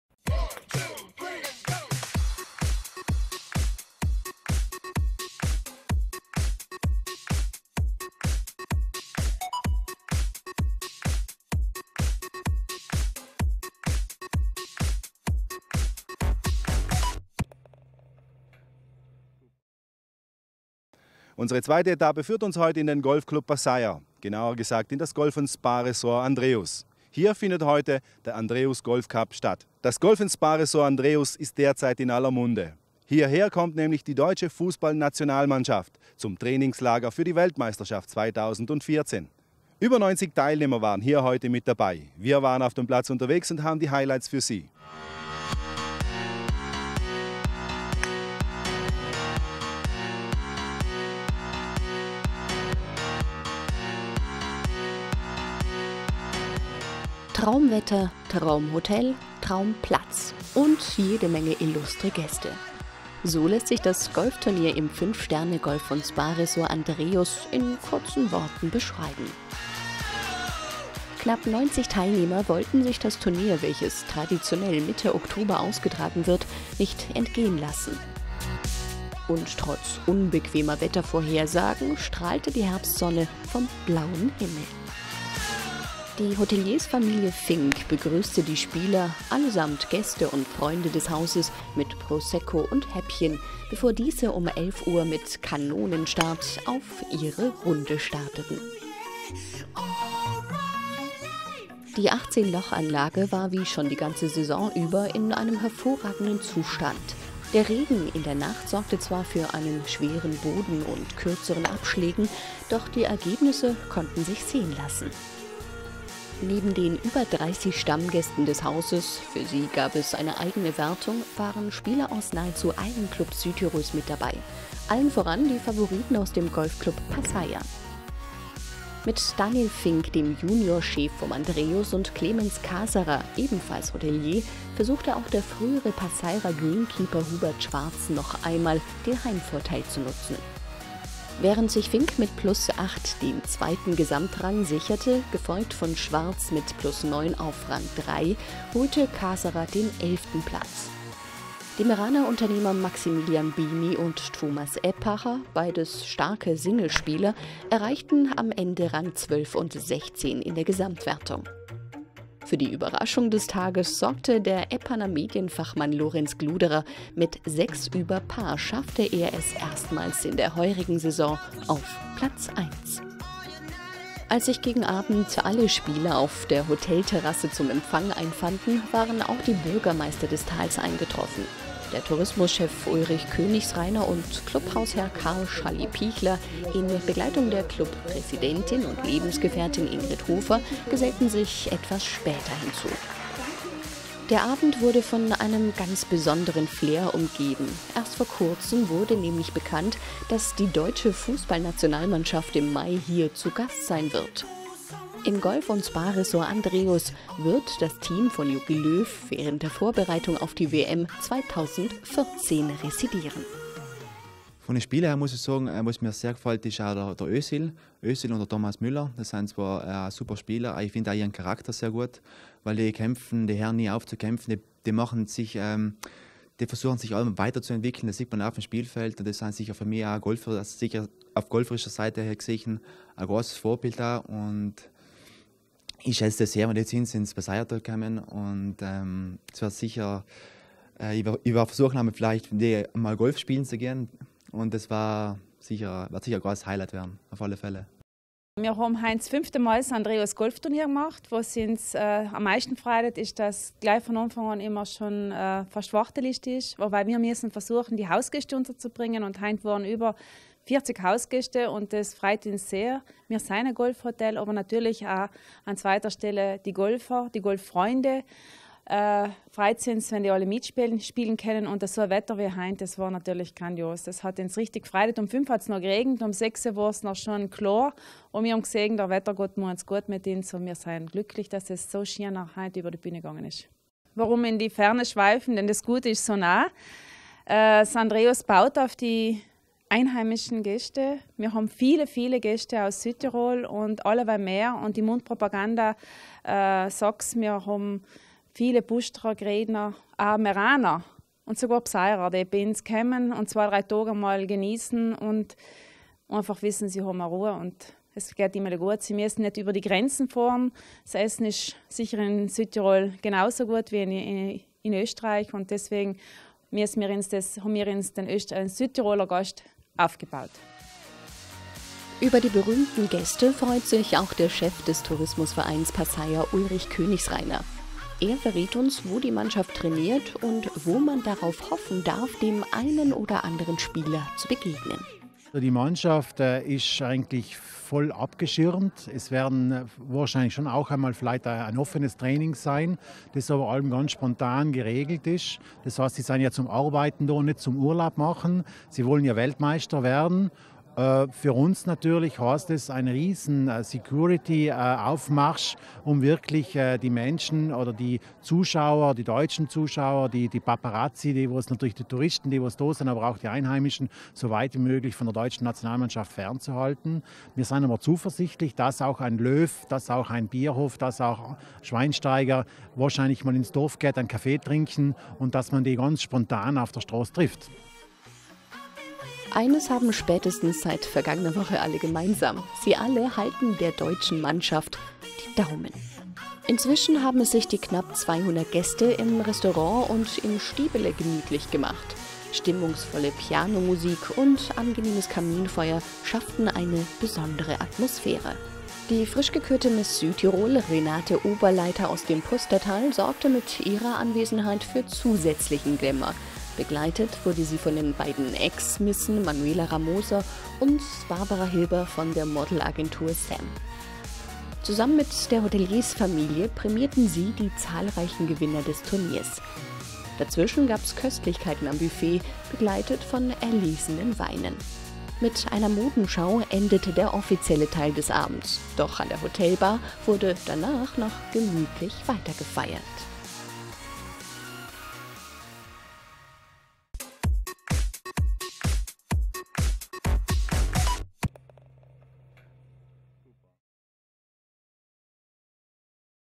5 Sterne Andreus Golfturnier 2013 am 18 Loch Golfplatz Passeier.Meran
Bereits zum 4 mal fand die ANDREUS Golftrophy des 5 Sterne Golf & Wellnesshotel Andreus bei Meran in Südtirol statt. Hausgäste, Golffreunde und Familie nahmen am Golfturnier teil. Wir freuen uns schon auf das Jubiläumsturnier 2014.